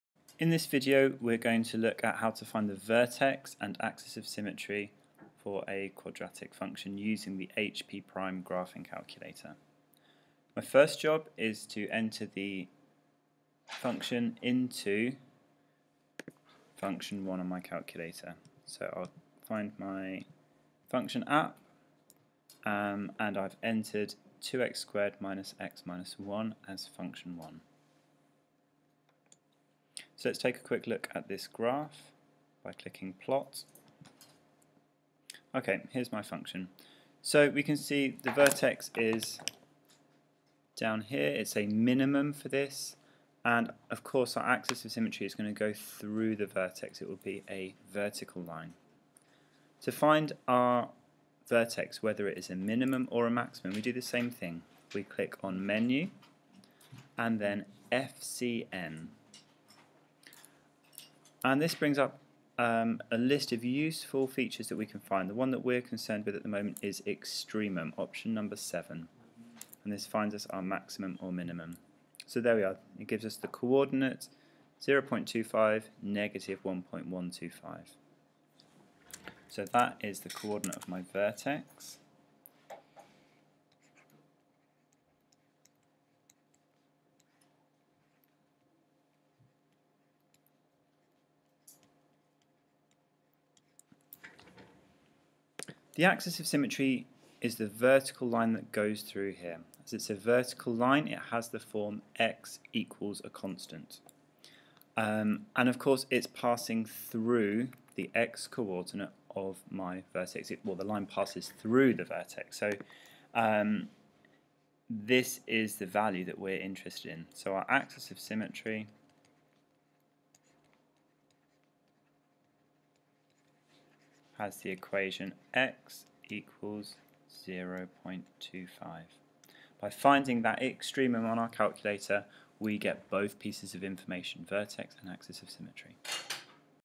HP Prime - Vertex and Axis of Symmetry of a Quadratic Function
Using the HP Prime Graphing Calculator to find the vertex and axis of symmetry of a quadratic function.